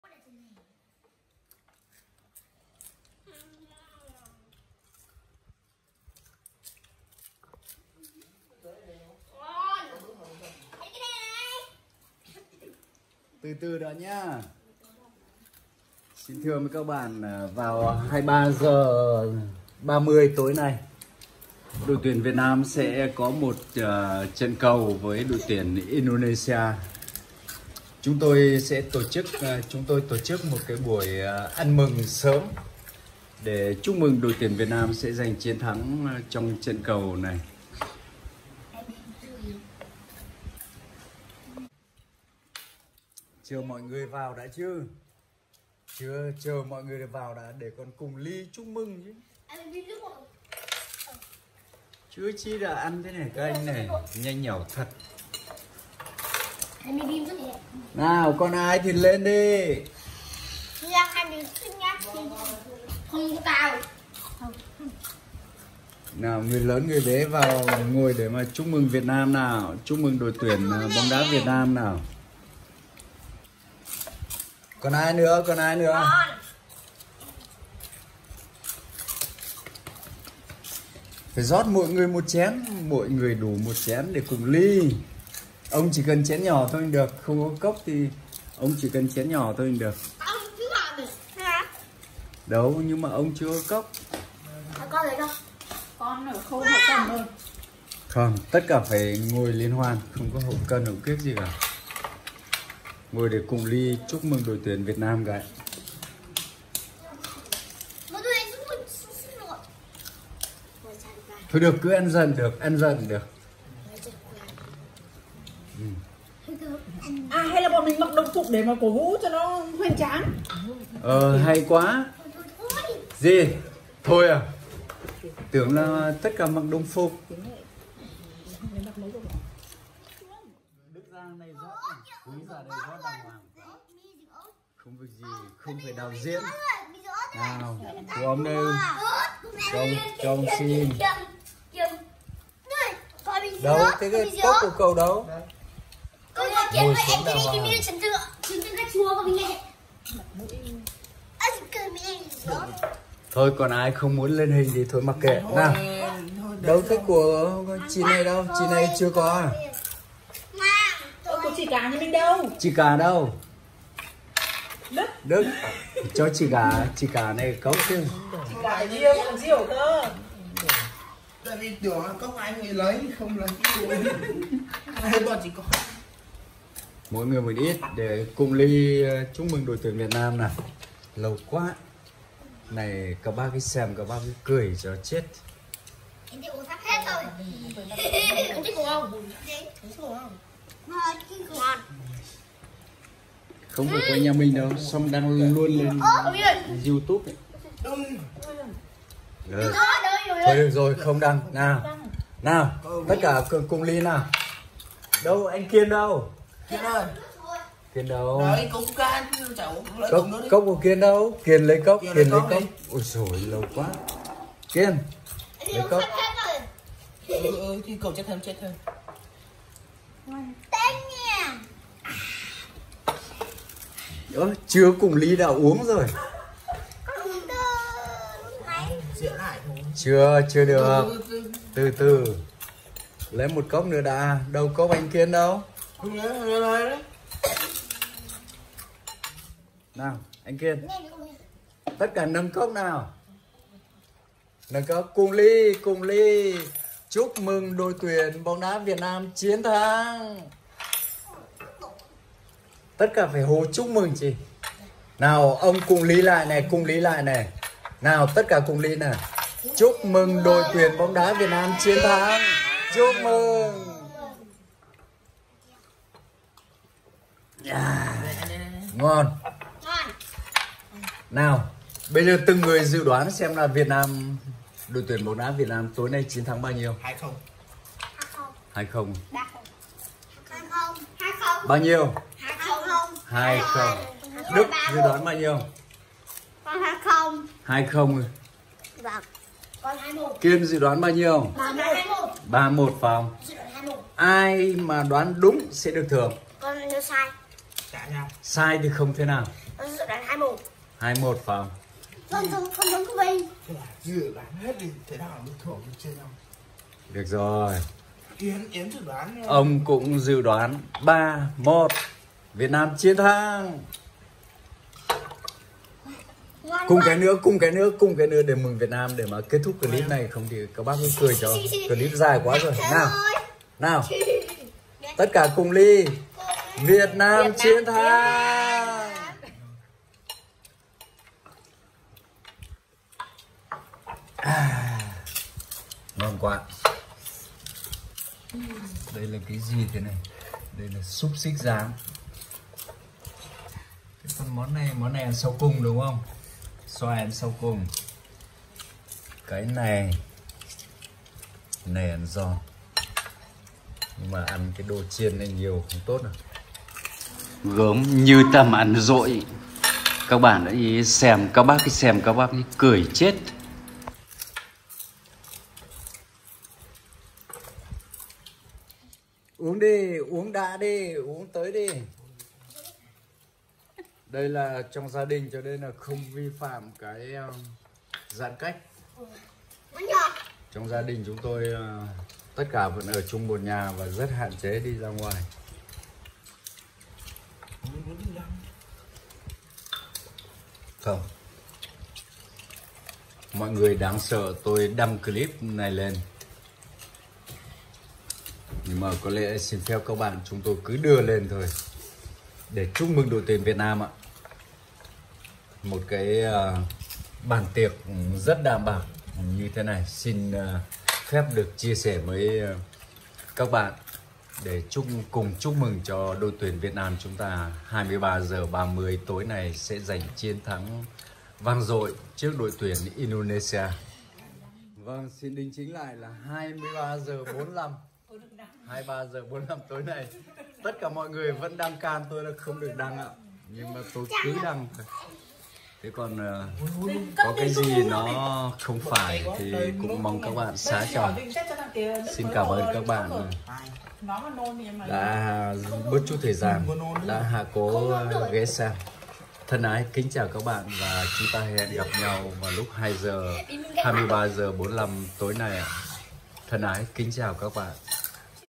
Ăn mừng sớm: Đội tuyển bóng đá Việt Nam Chiến - Thắng!
Chập tối tổ chức ăn mừng bóng đá cho các bạn nhỏ...!